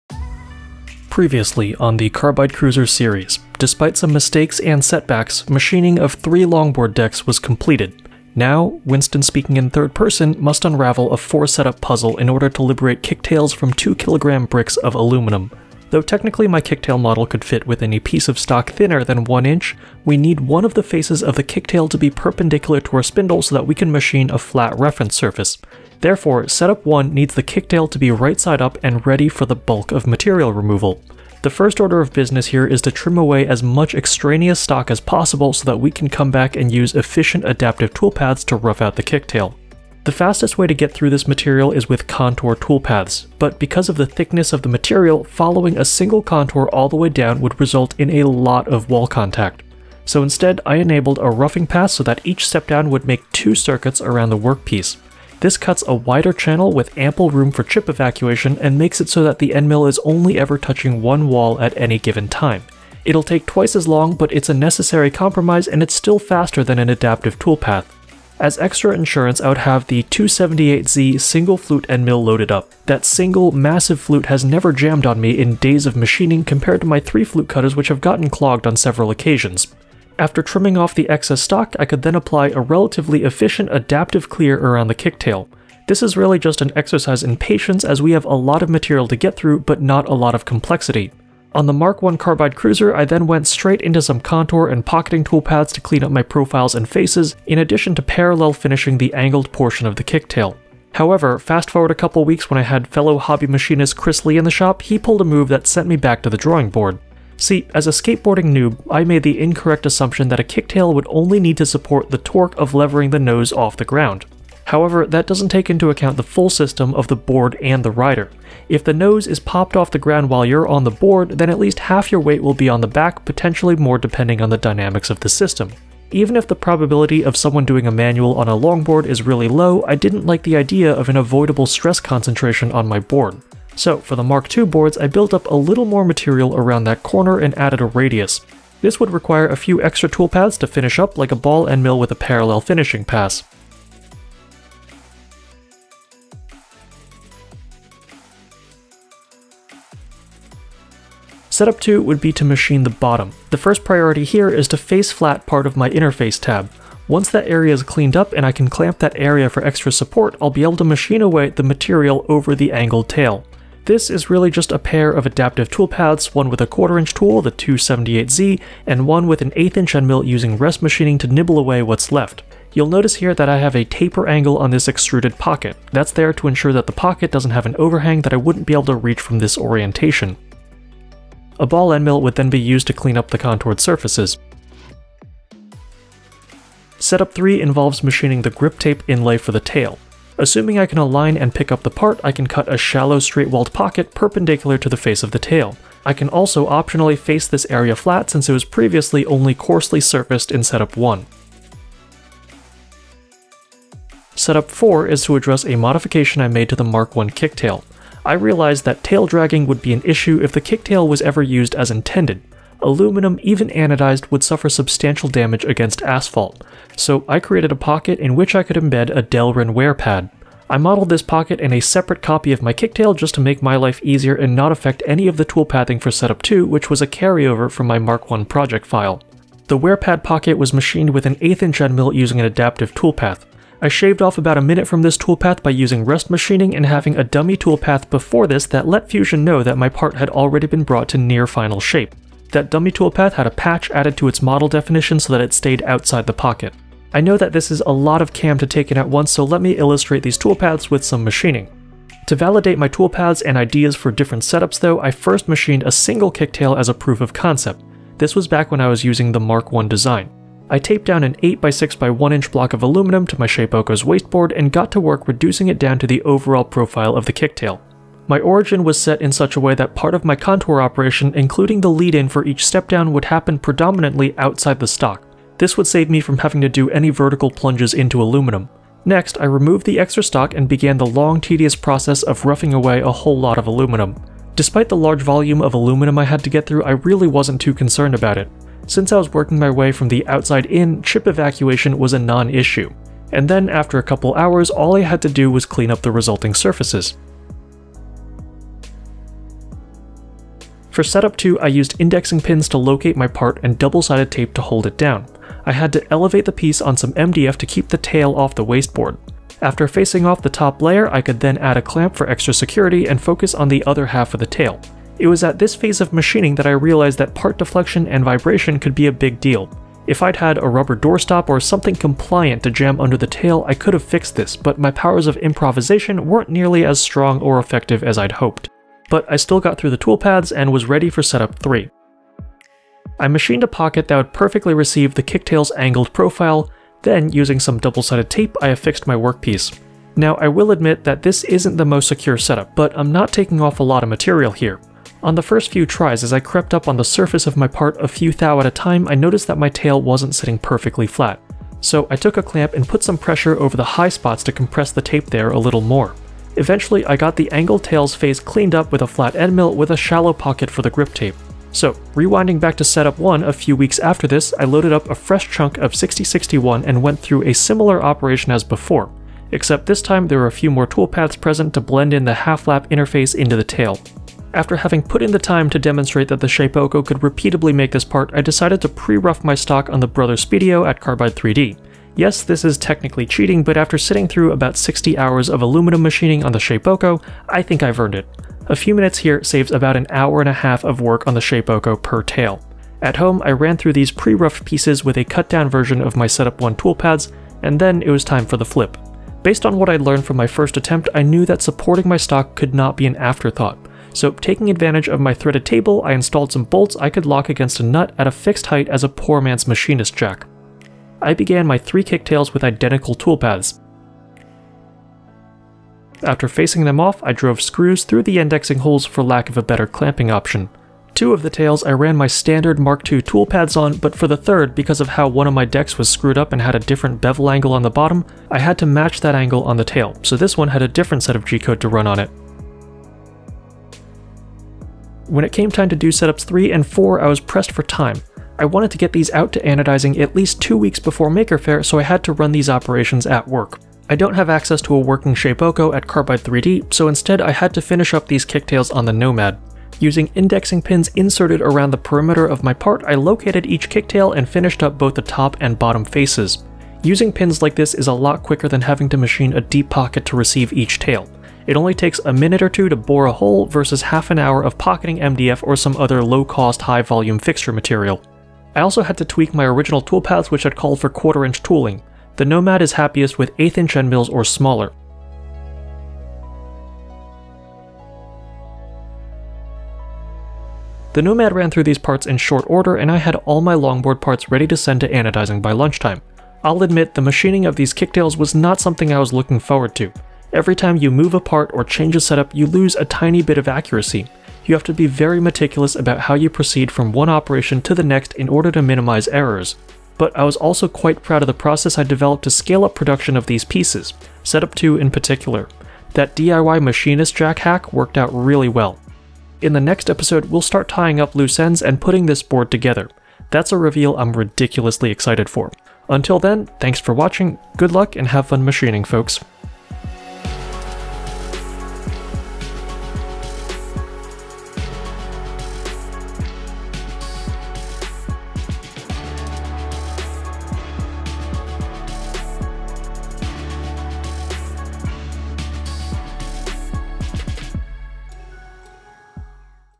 Machining Aluminum Longboard Kicktails - Carbide Cruiser Ep. 4
It's finally time to tackle the last structural piece of the Carbide Cruiser, the tail. This angled features provides maneuverability options for street riding as well as a distinctive visual signature that sets this board apart from other longboards.

This project was machined on the Shapeoko 3 XL. To learn more about our line of CNC milling machines, check out